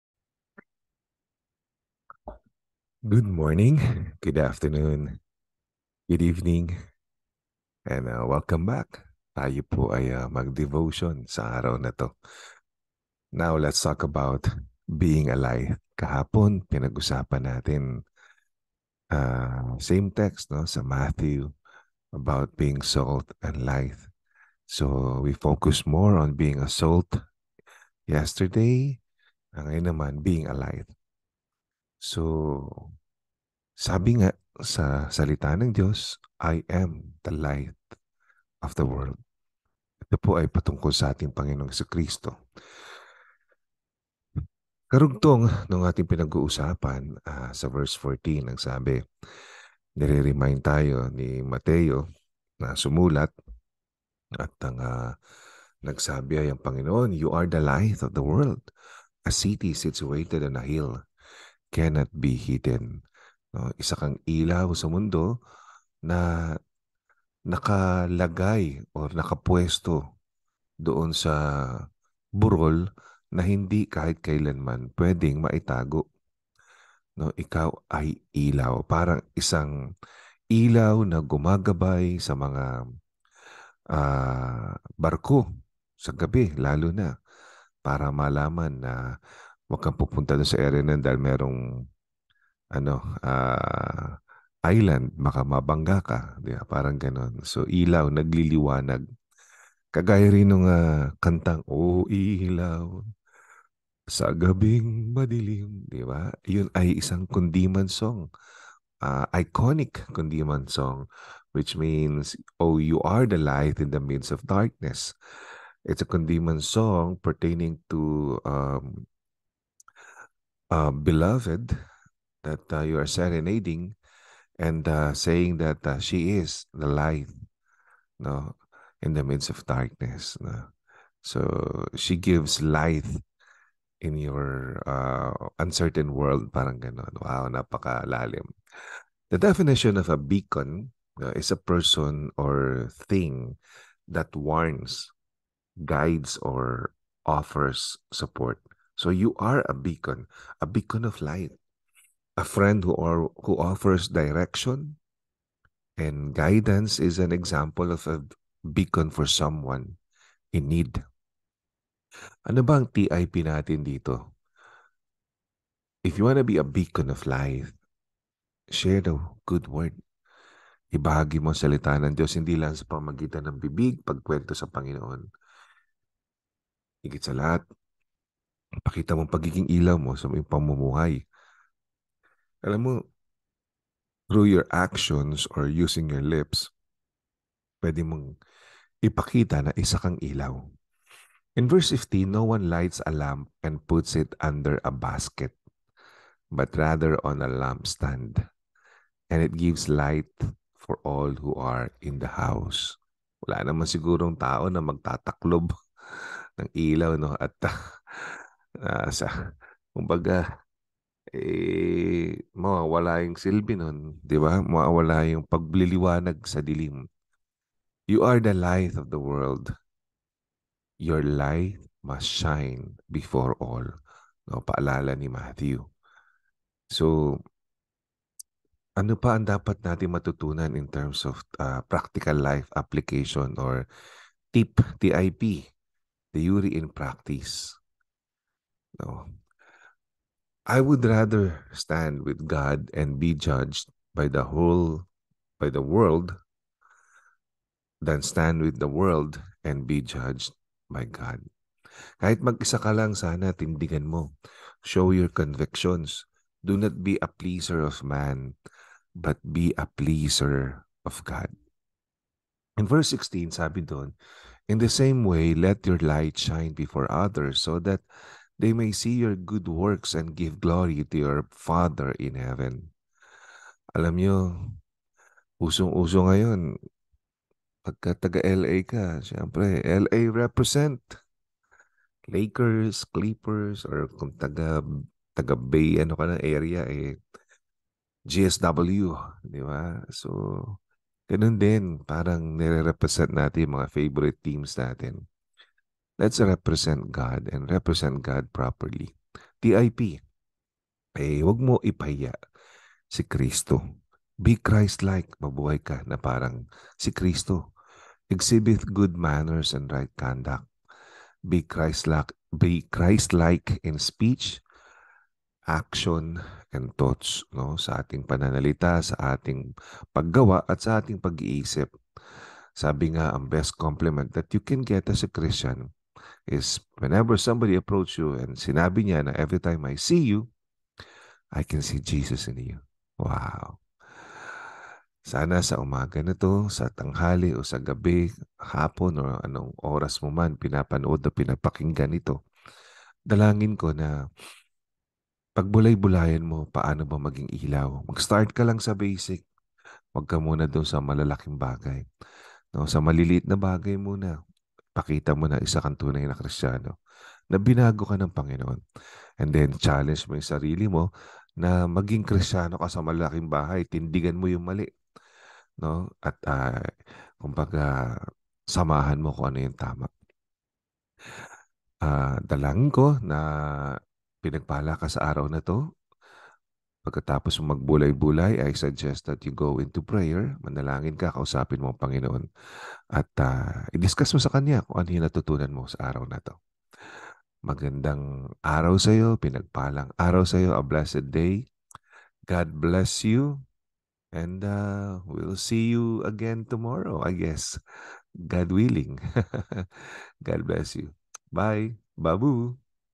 6th Daily Devotion You are the Light of the World Matthew 5:14-16 (Jeff Padre Guiwa)
6th Daily Devotion You are the Light of the World Matthew 5:14-16
(Jeff Padre Guiwa)

LAFILNAZ
SIMBANG PINOY